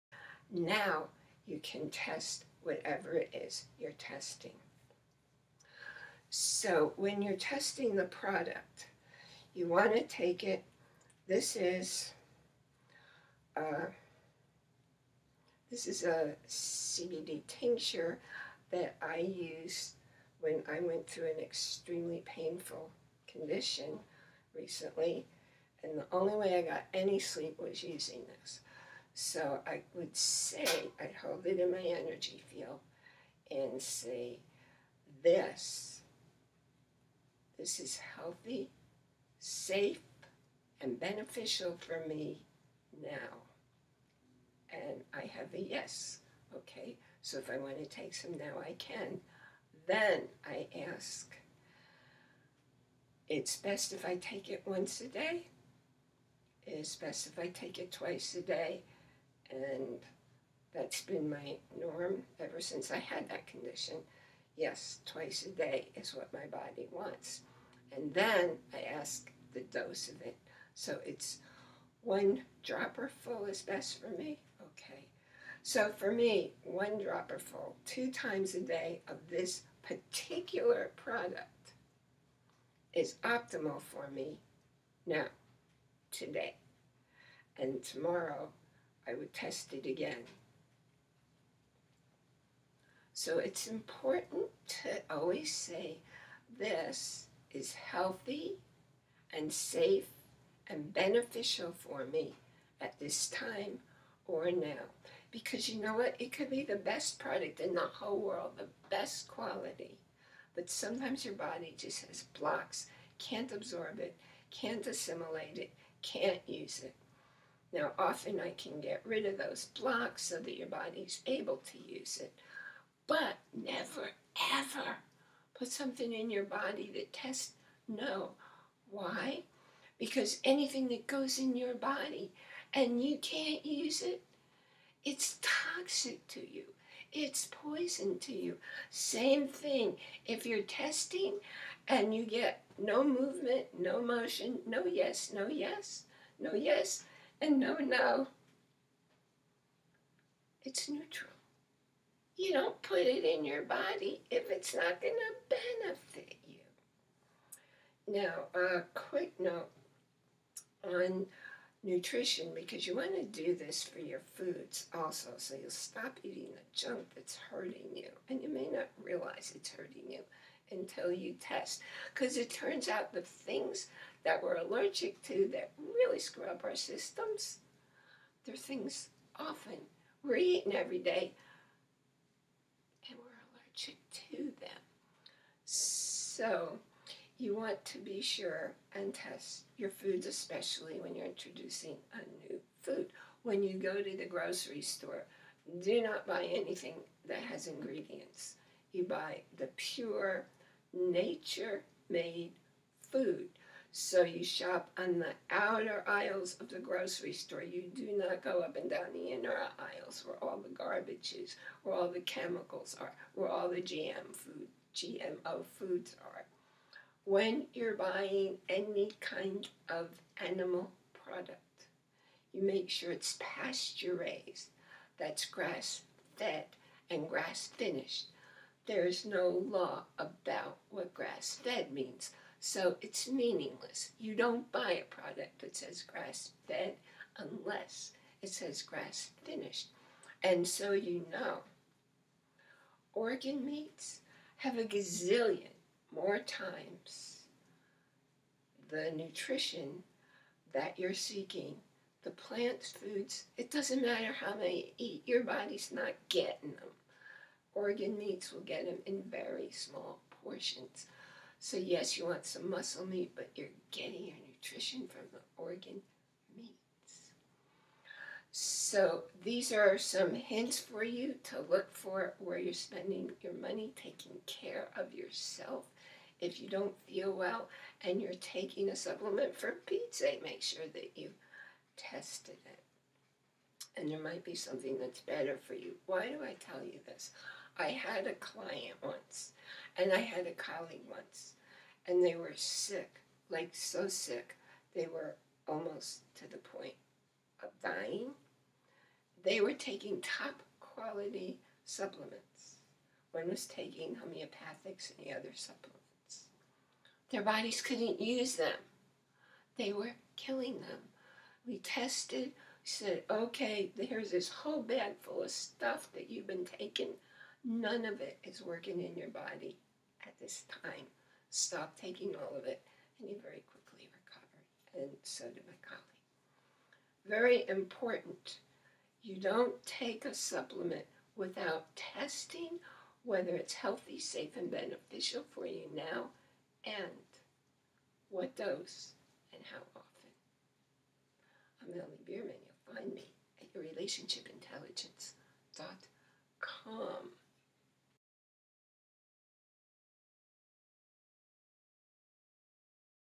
How To Test Your Supplements
Before taking any medication or supplements or trying a new food it is wise to test yourself to be sure it is safe for you. Test for safety, efficacy, dose and frequency.
Anything you ingest that fails to benefit you may harm you without your awareness.